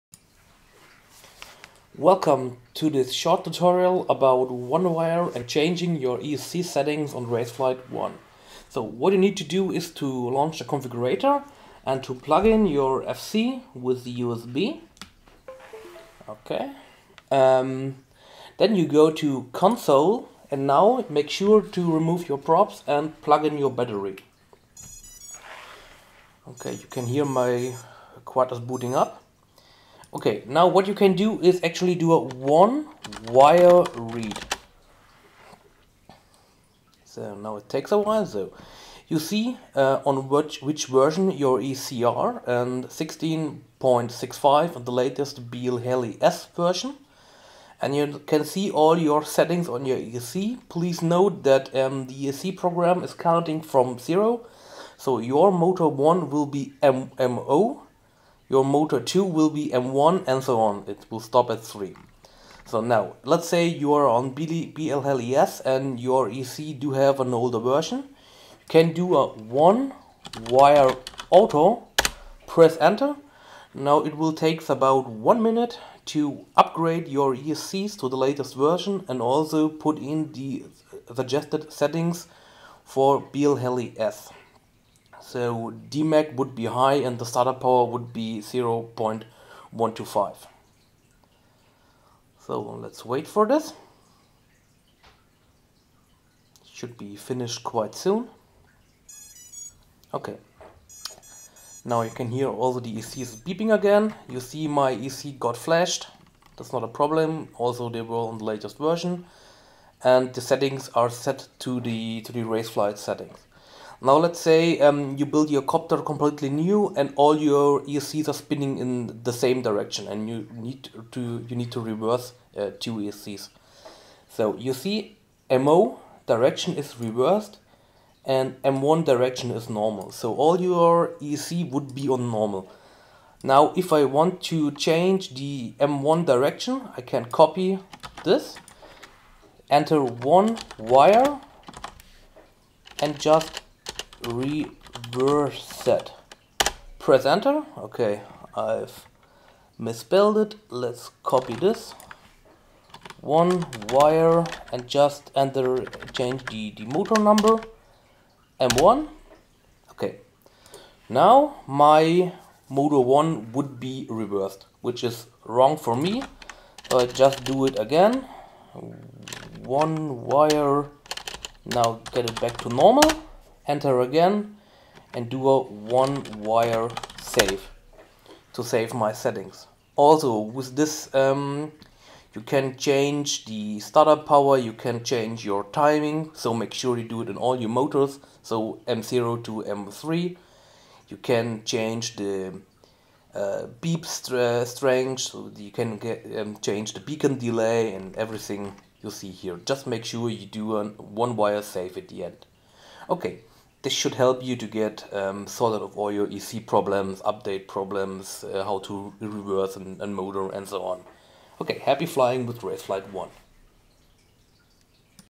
[ENG] Raceflight RF1 - 1wire ESC tutorial
Quick 1wire tutorial about changing settings on your esc or to upgrade them.

Please make sure to remove your props